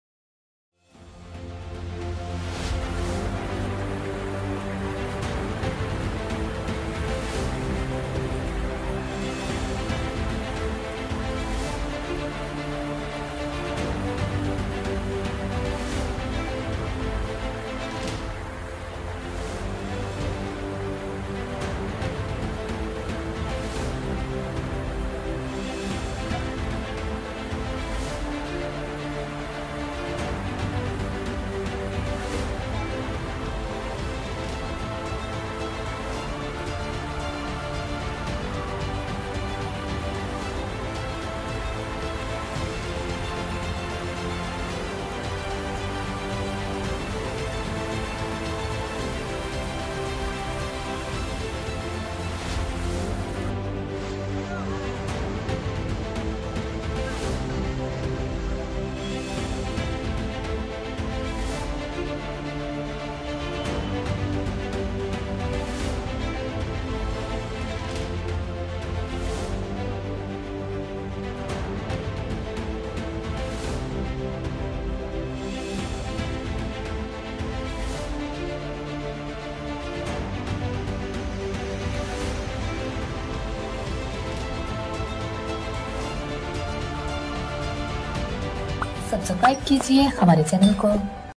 Telangana Kolkata rain | Waterlogging - worldnews24
Telangana । Several roads waterlogged in Sircilla town due to incessant rainfall in the area. Waterlogging was observed in many parts of Kolkata city after the rains. The video is from South Kolkata area.